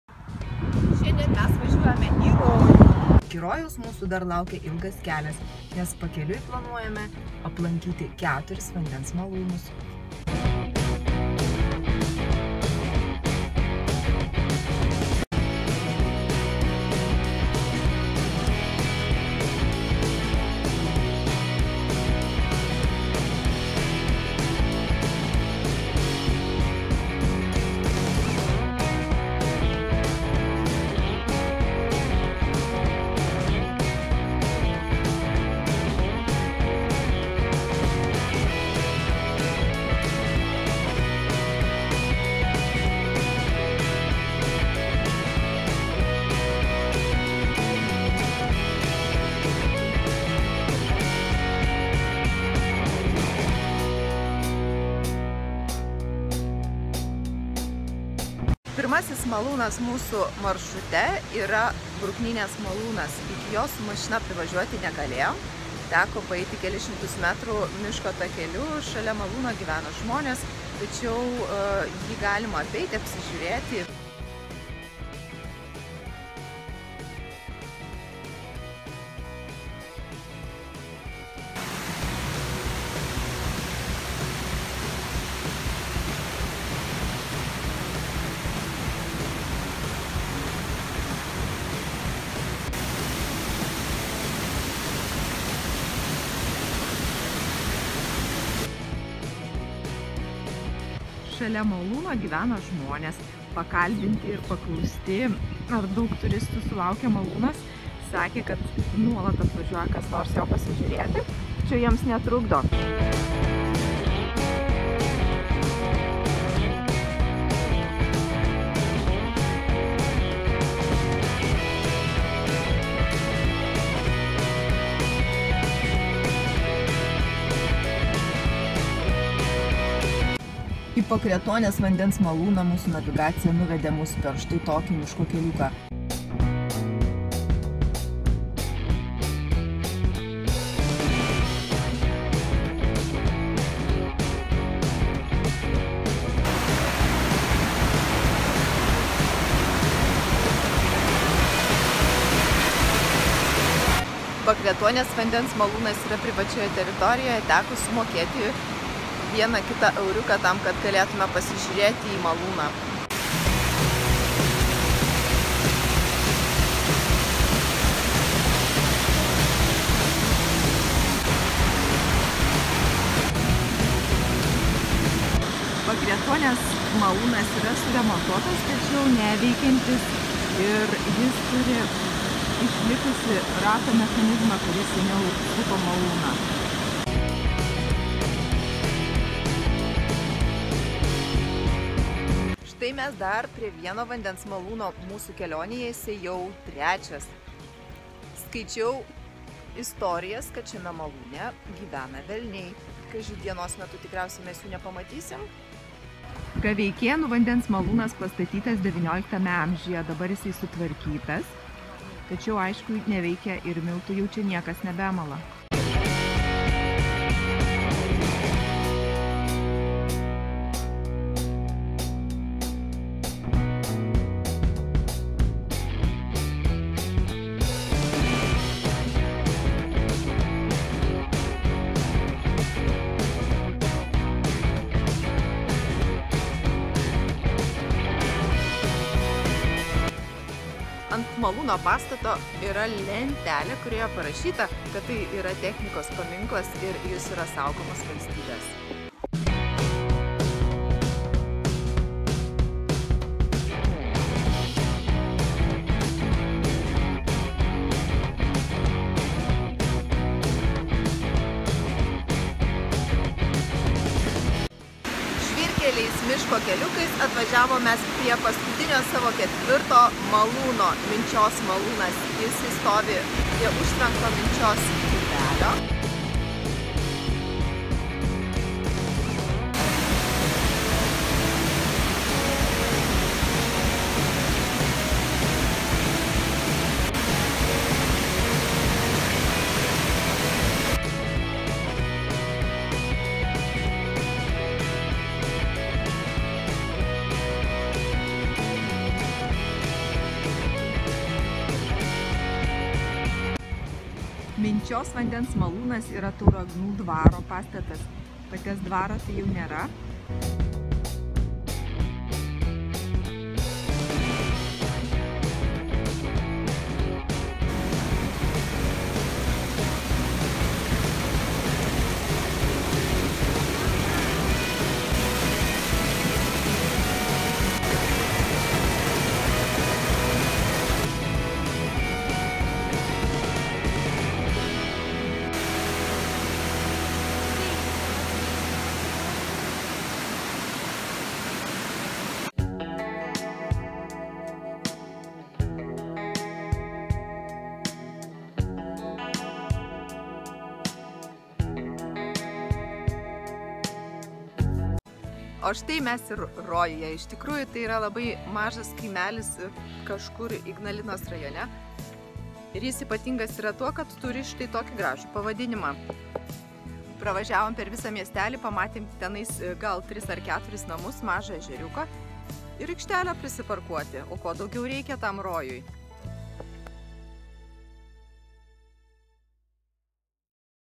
Traveling in Lithuania || Long road to Heaven || VLOG #6
Žydras be debesėlio dangus ir nuo pat ryto skaisčiai šviečianti saulė žadėjo gražią dieną. Dieną puikiai tinkamą mūsų suplanuotai kelionei į Rojų. Tačiau iki šios gražiai pavadintos vietos mūsų laukė dar, kaip vėliau patyrėme, ilgas kelias, nes pakeliui ketinome rasti ir apžiūrėti net keturis vandens malūnus.

Aplankytų malūnų koordinatės:
Bruknynės vandens malūnas - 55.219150, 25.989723
Pakretuonės vandens malūnas - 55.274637, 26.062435
Gaveikėnų vandens malūnas - 55.34954; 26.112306
Minčios vandens malūnas - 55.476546, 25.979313

Prenumeruokite Check in Lithuania Youtube kanalą, kad visada pirmieji sužinotumėte apie mūsų naujus nuotykius ir įdomias lankytinas Lietuvos vietas.

Būkit aktyvūs, būkit smalsūs, būkit laimingi!

Be active, be curious, be happy!